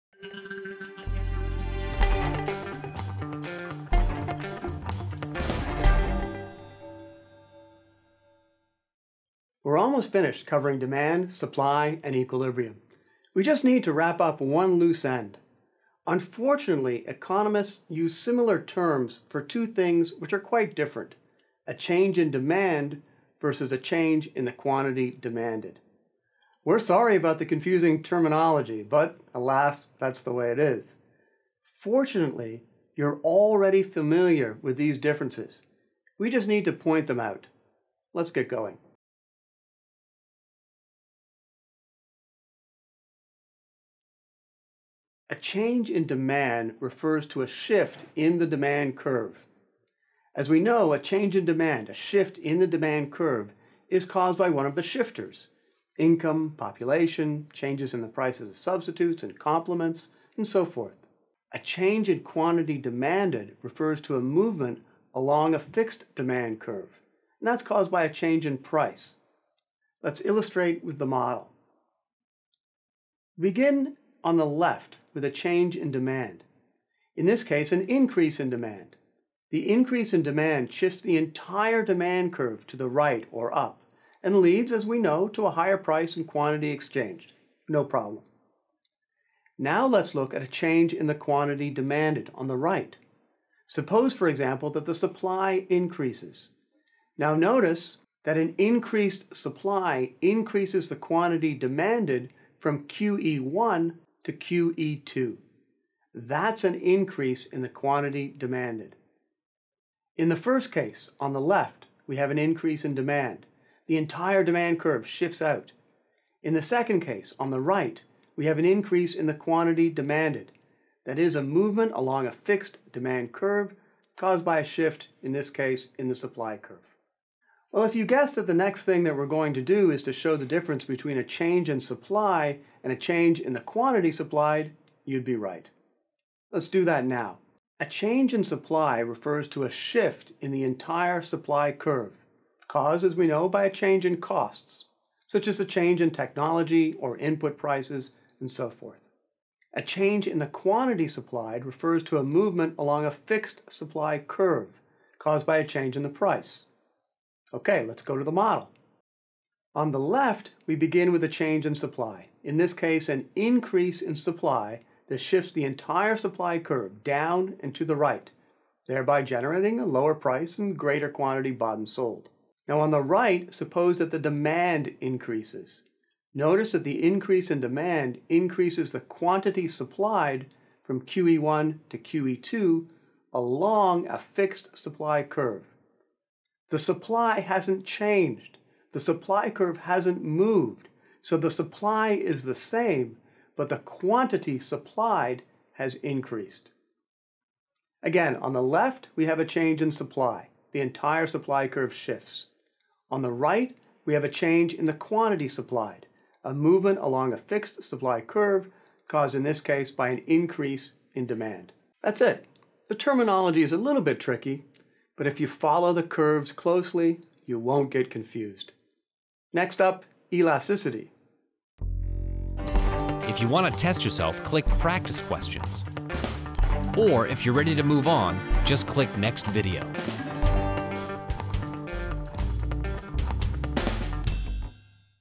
Supply and Demand Terminology
What is the difference between a change in demand and a change in the quantity demanded? The terminology can be confusing — but we’ll provide some clarity in this video. In short, a change in demand refers to a shift in the demand curve — caused by a number of factors such as income, population, etc. A change in quantity demanded refers to a movement along a fixed demand curve — caused by a change in price.

Econ In The News, a free weekly email of class-ready news articles

00:00 Introduction
00:44 Change in Demand vs. Change in Quantity Demanded
01:12 Graph Example Comparison (Demand, Quantity Demanded)
02:07 Change in Supply vs. Change in Quantity Supplied
02:38 Graph Example Comparison (Supply, Quantity Supplied)